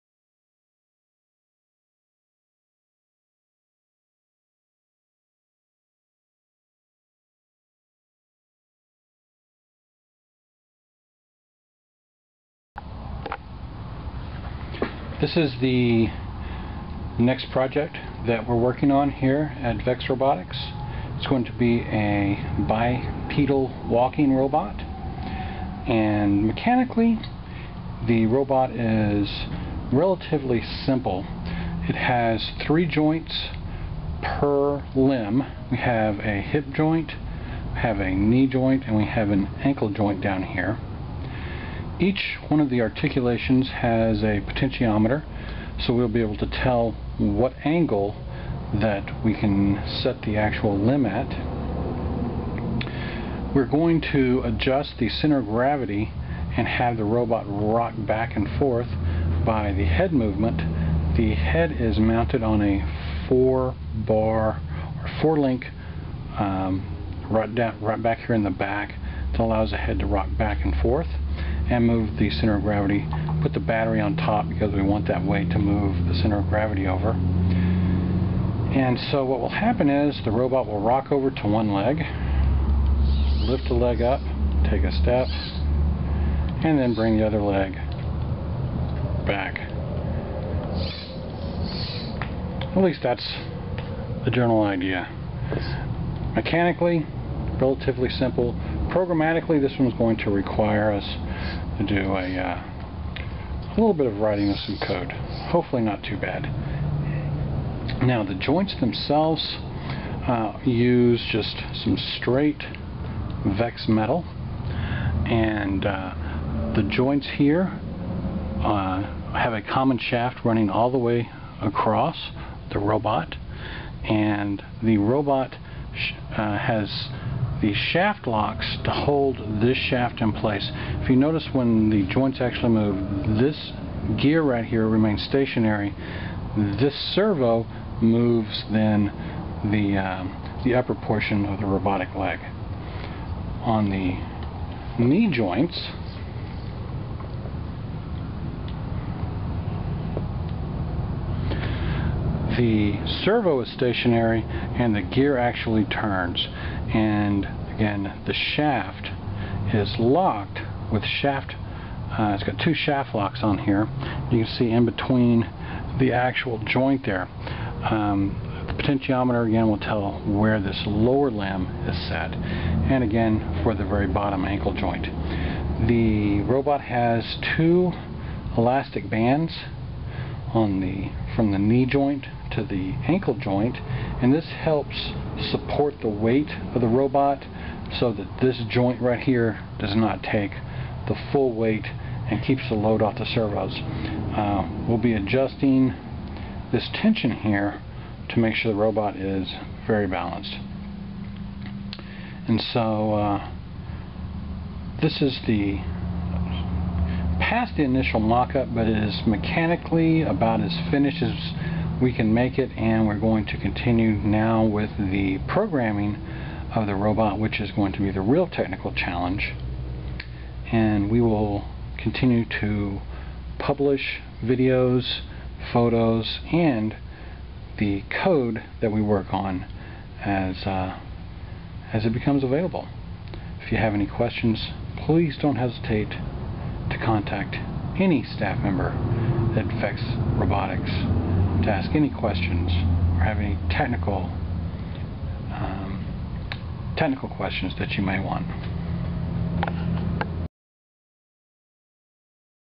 Vex Walking Robot - Model I
VexRobotics is currently working on a Bipedal Walking Robot.  This is the initial mechanical demonstration.